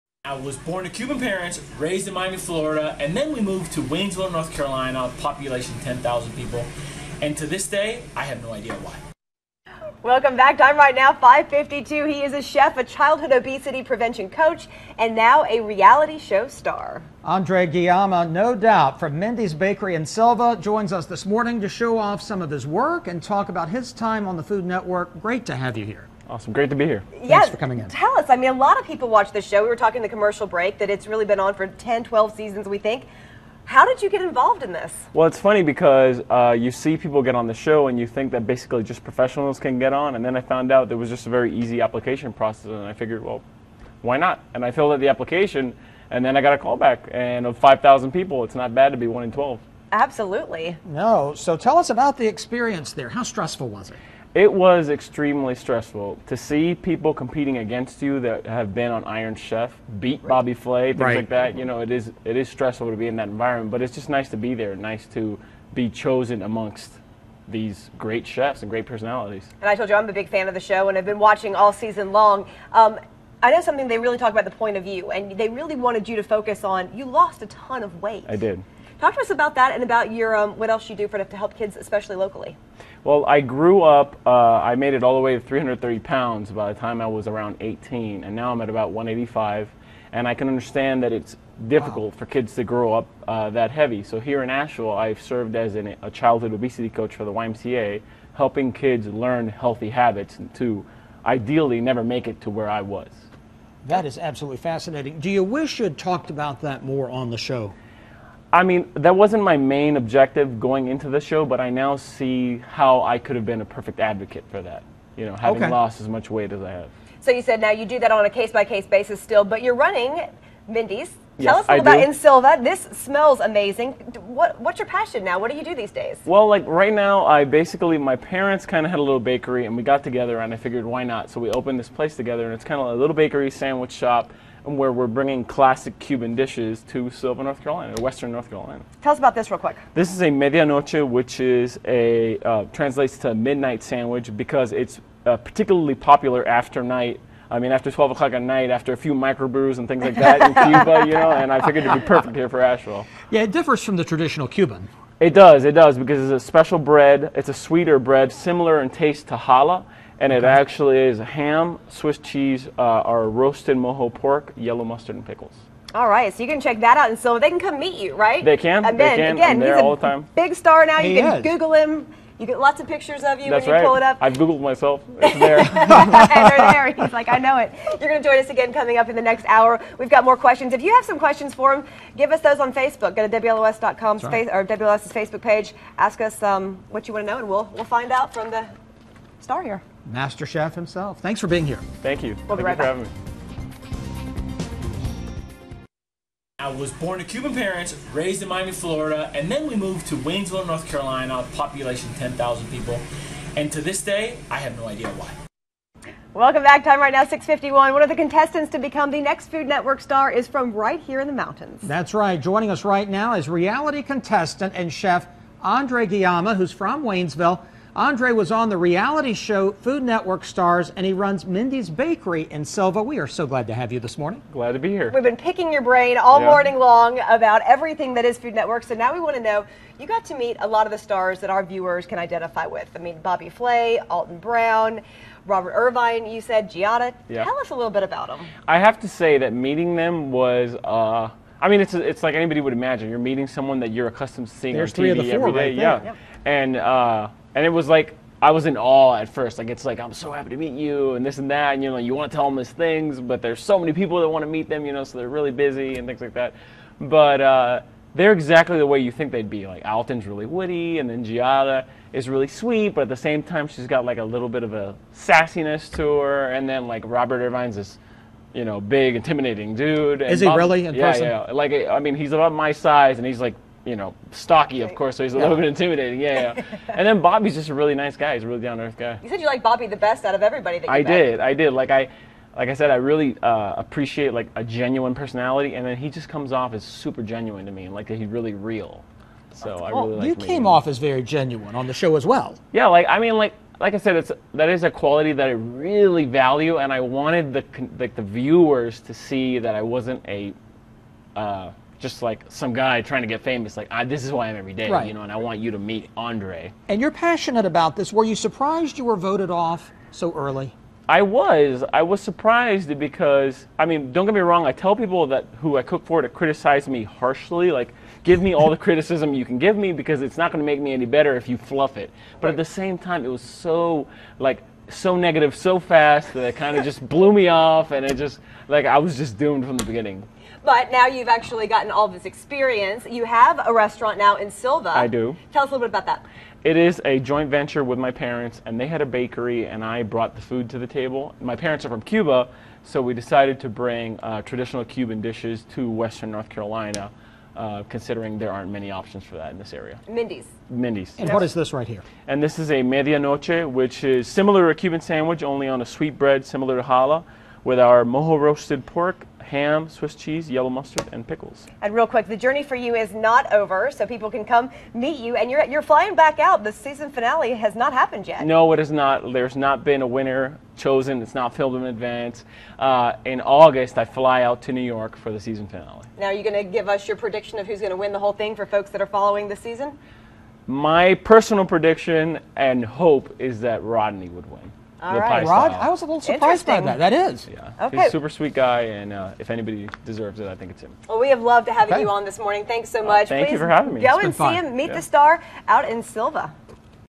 Food Network Star Visits
Andres Guillama is a finalist on Season 9 of Food Network Star. He stopped by News 13 This Morning to show off some of his work and talk about his time on the Food Network.

Andres, 26, grew up in a traditional Cuban family and worked at his father's restaurant. After losing 150 pounds, Andres became a childhood obesity prevention coach, and is now eager to show viewers how deliciously satisfying healthy food can be.

Andrews works at Mindy's Bakery in Sylva.